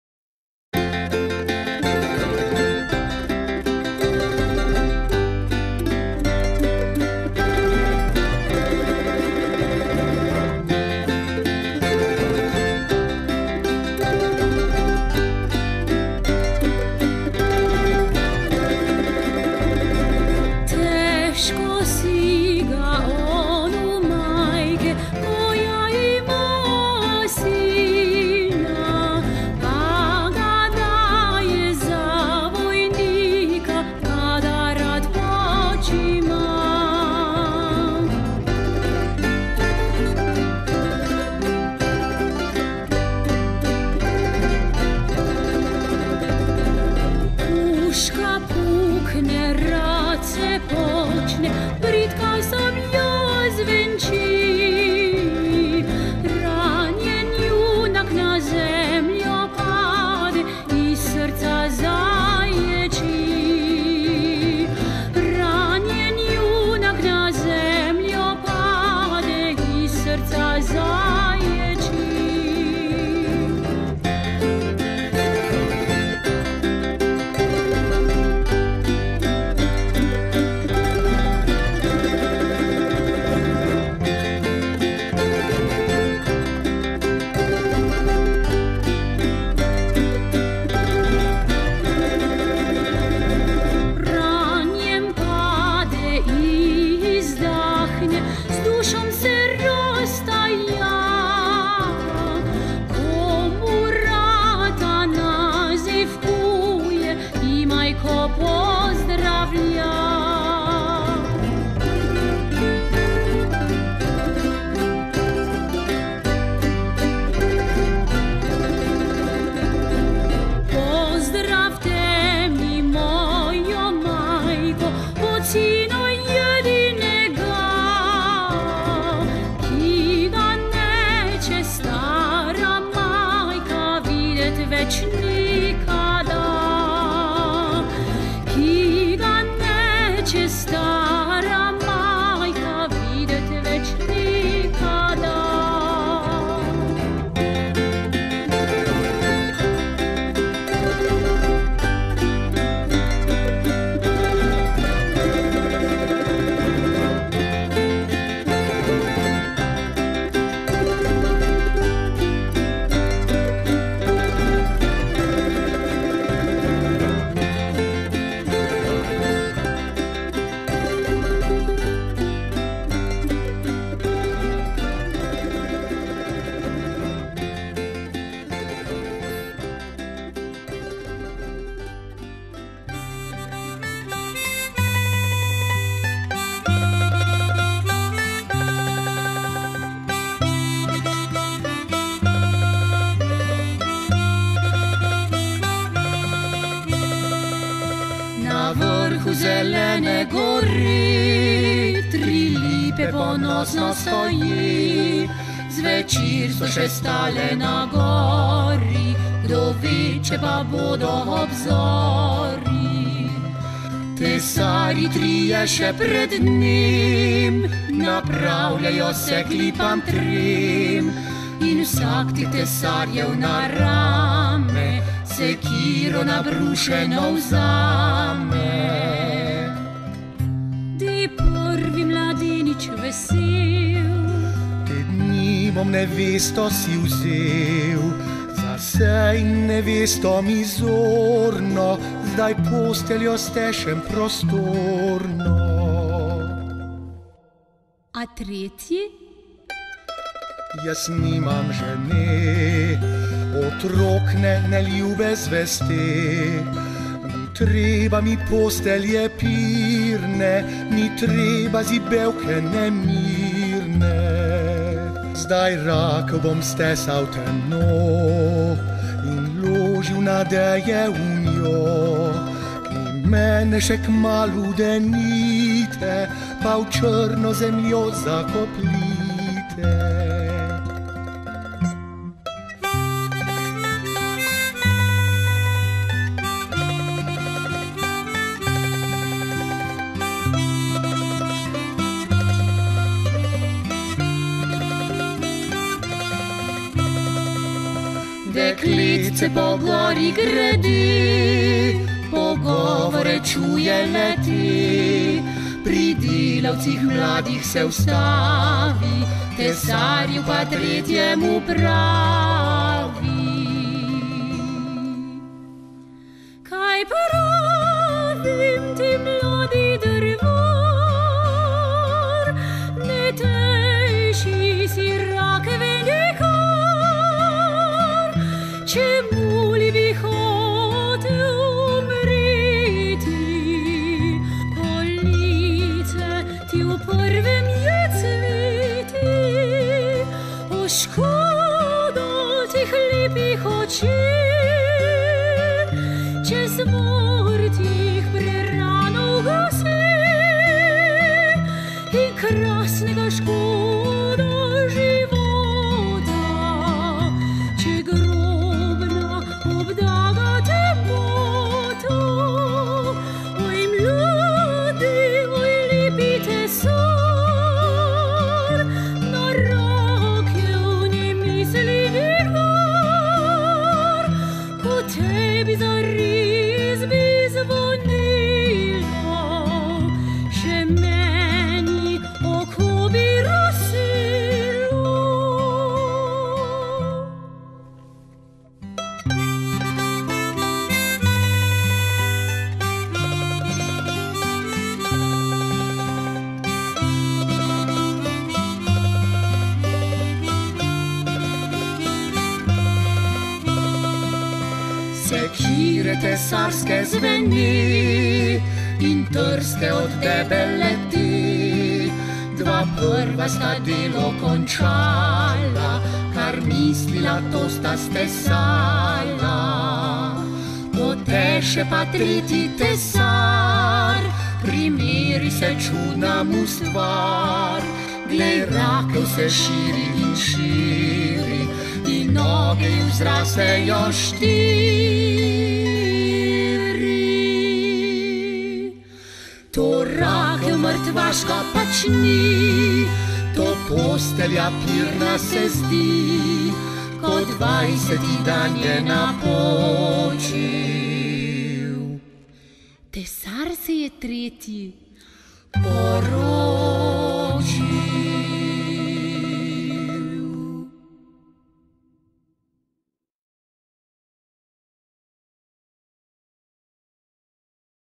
Tehnični dan učencev 7. razreda
Ogled razstave Alojz Gangl - kipar na poti v moderno (Galerija Kambič Metlika) in delavnica v organizaciji Belokranjskega muzeja Metlika.